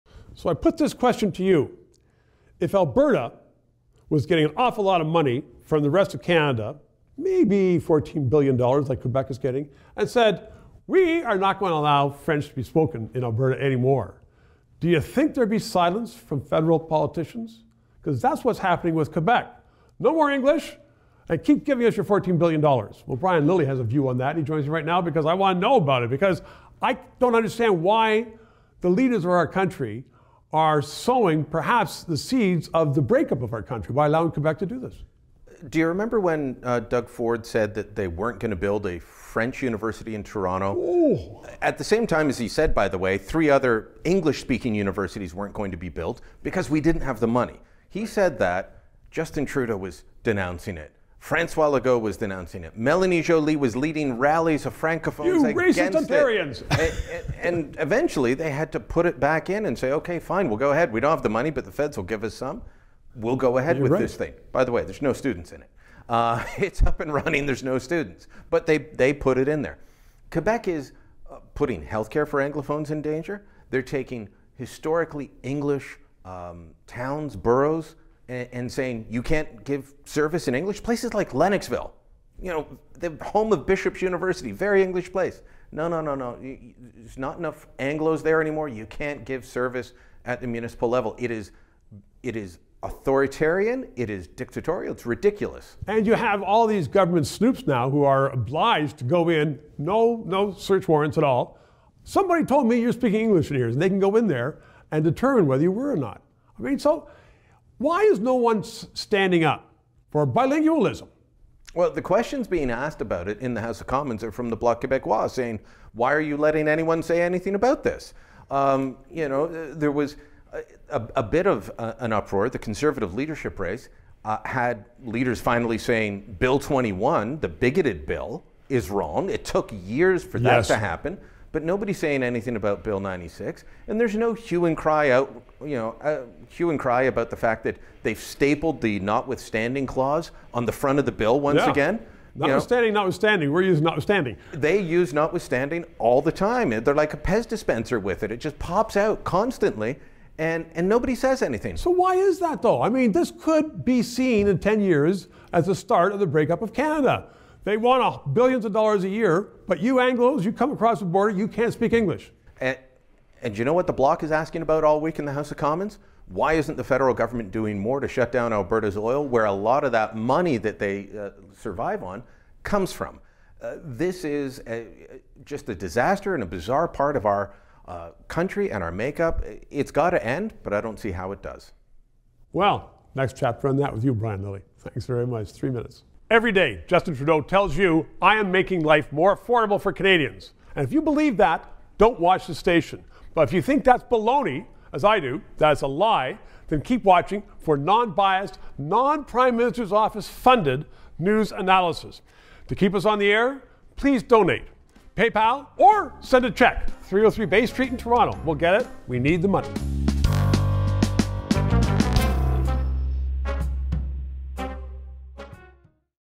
Justin Trudeau Is Allowing Quebec To Break Up Canada
If Alberta, or British Columbia announced they were going to serve its citizen in only english, and do away with french - would the politicians in Ottawa remain silent? Why are the Justin Trudeau Liberals remaining silent as Quebec is eliminating English services - even in traditionally English parts of the province. Brian Lilley is a columnist with the Toronto Sun, he joins Stephen LeDrew to discuss this for Three Minutes.

Cheques accepted at:
Stephen LeDrew
303 Bay Street
Toronto, Ontario
M5H 2R1